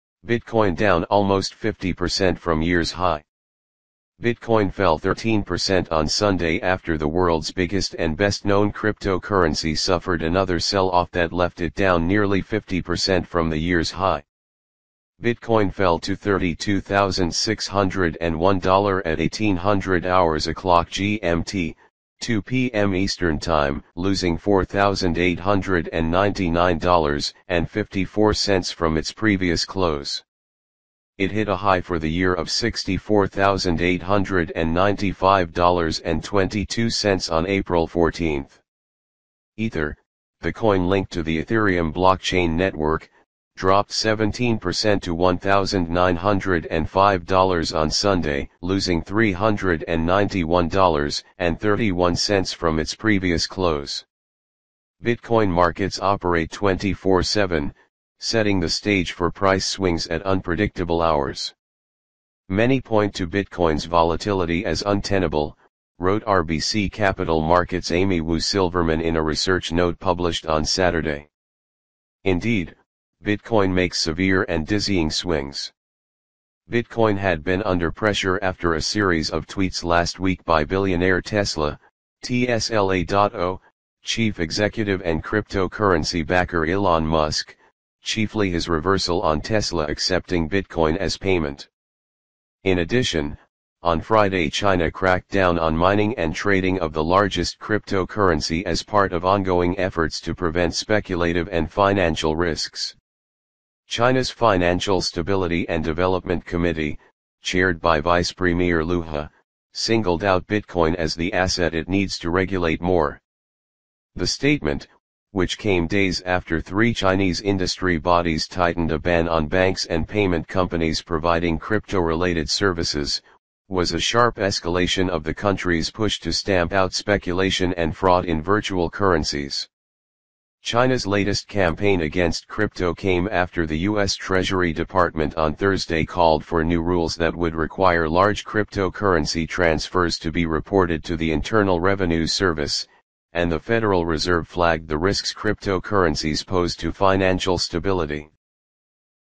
Bitcoin Down Almost 50% From Year's High
Bitcoin fell 13% on Sunday after the world's biggest and best-known cryptocurrency suffered another sell-off that left it down nearly 50% from the year's high.

Bitcoin fell to $32,601 at 1800 GMT (2 p.m. ET), losing $4,899.54 from its previous close. It hit a high for the year of $64,895.22 on April 14.

Ether , the coin linked to the ethereum blockchain network, dropped 17% to $1,905 on Sunday, losing $391.31 from its previous close.

Bitcoin markets operate 24/7, setting the stage for price swings at unpredictable hours.